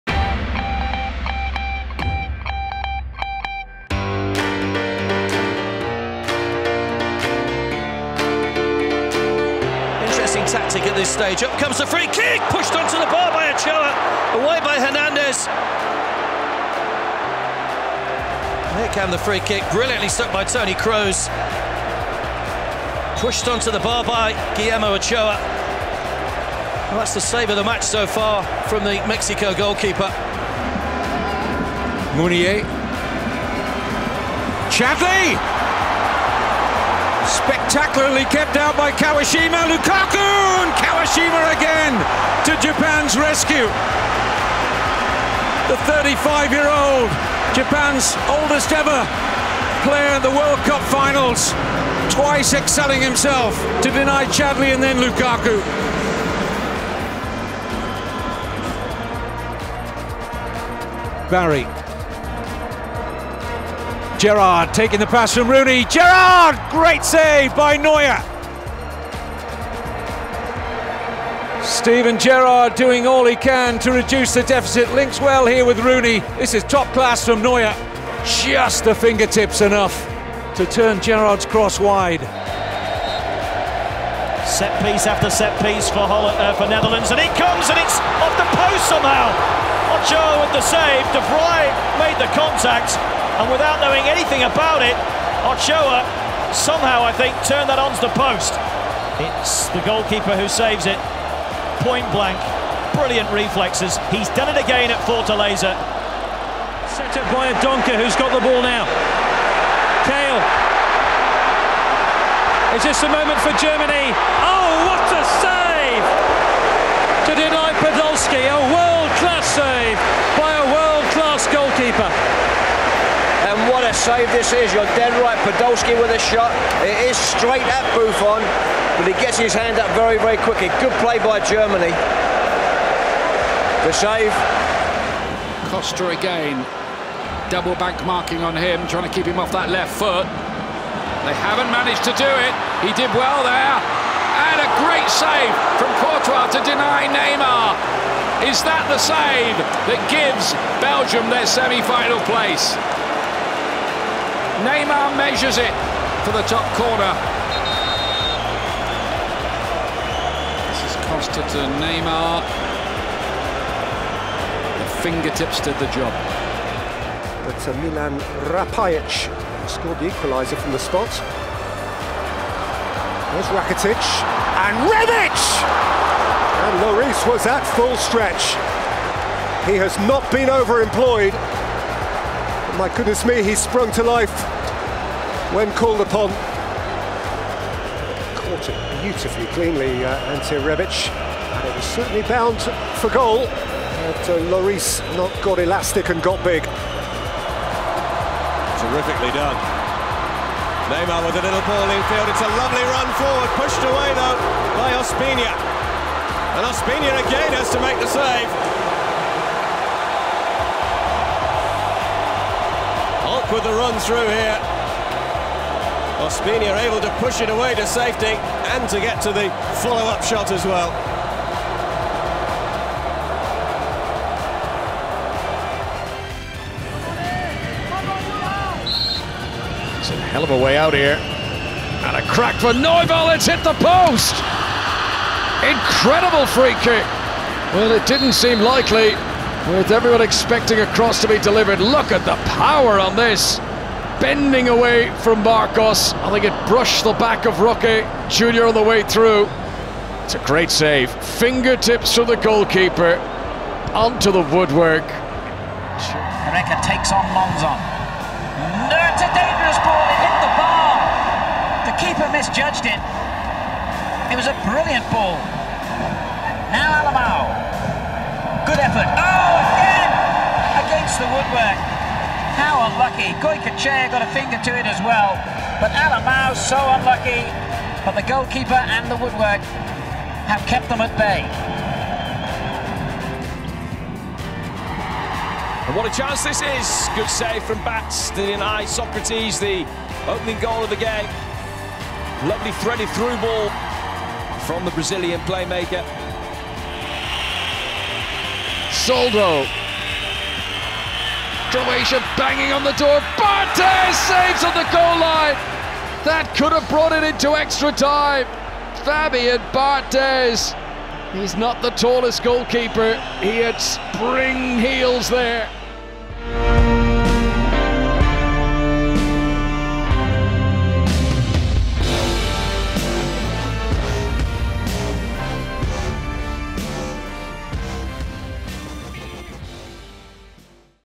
WorldCupAtHome | Unbelievable Saves | Neuer, Buffon, Lloris
This one's for the goalkeepers! Relive stand-out saves by world class stoppers from WorldCupAtHome matches.

Follow our WorldCupAtHome playlist on Spotify to relive some of the best songs from the FIFA World Cup wonder years